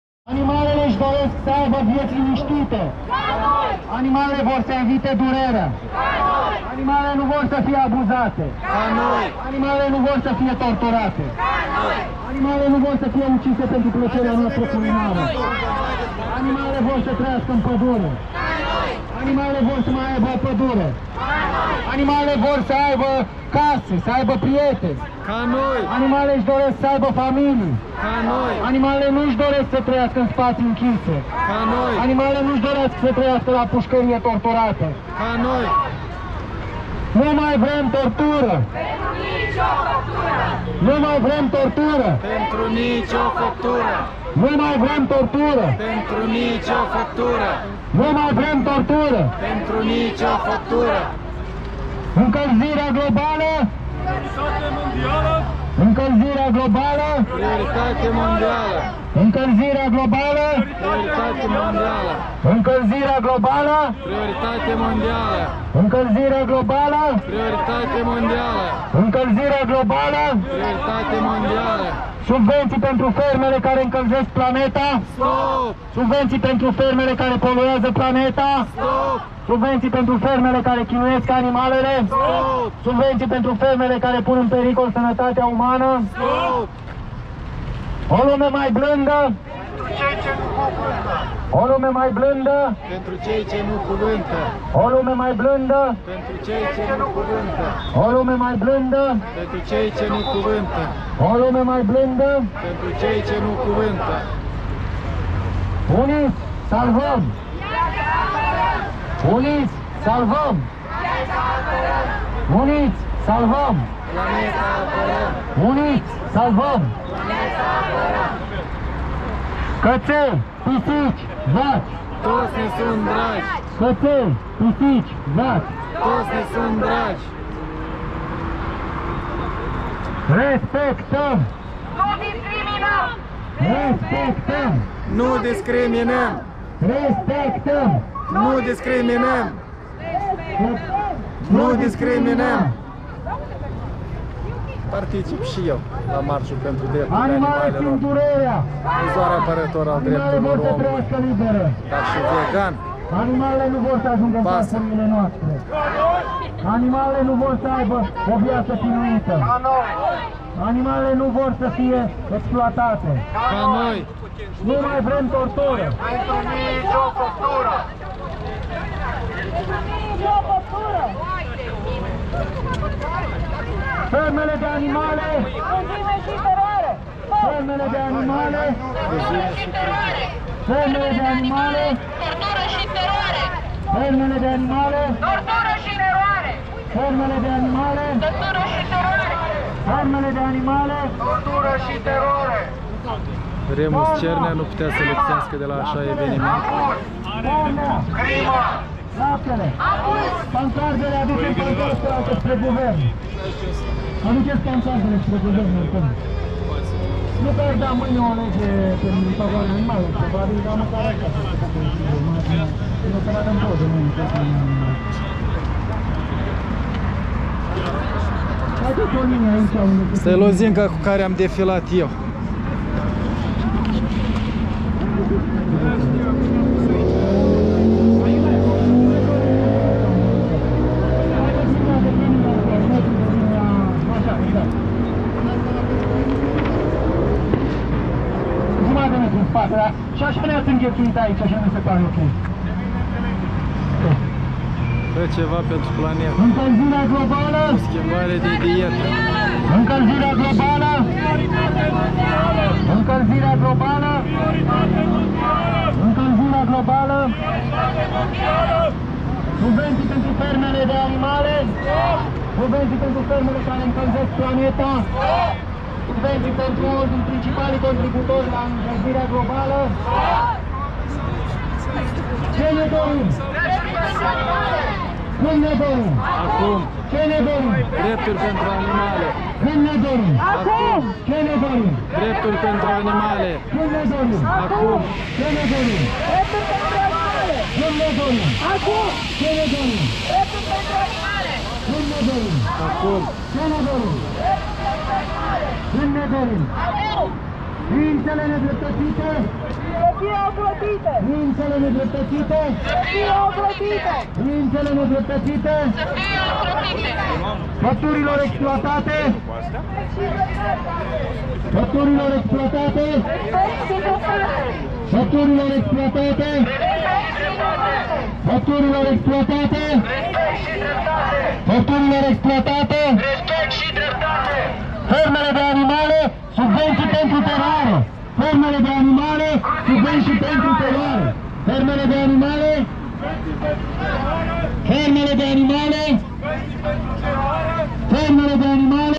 Marș vegan spre Guvernul României - Curaj.TV
Vegani și iubitori de animale, am protestat azi împotriva abuzurilor și exterminării animalelor cu subvenții de la stat. Am mers în marș de la Universitate pînă în Piața Victoriei. Nicio televiziune n-a relatat de la eveniment